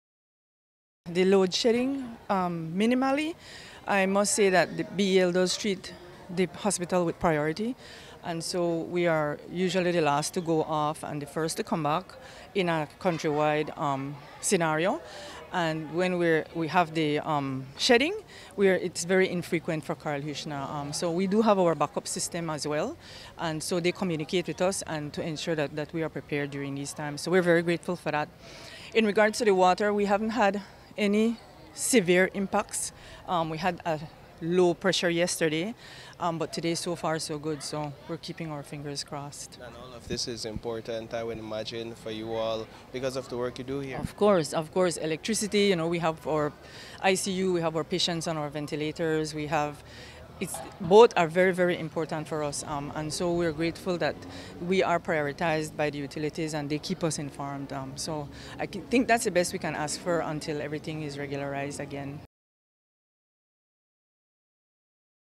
K.H.M.H. Prioritized by Utility Companies
When we caught up with Chandra Cansino, the Chief Executive Officer at the Karl Heusner Memorial Hospital, earlier today, we asked her if B.E.L.’s loadshedding and B.W.S.L.’s water situation have had any negative impacts on the hospital’s services. She says the utility companies prioritize the healthcare facilities, ensuring that they remain with light and water for as long as possible under any circumstance.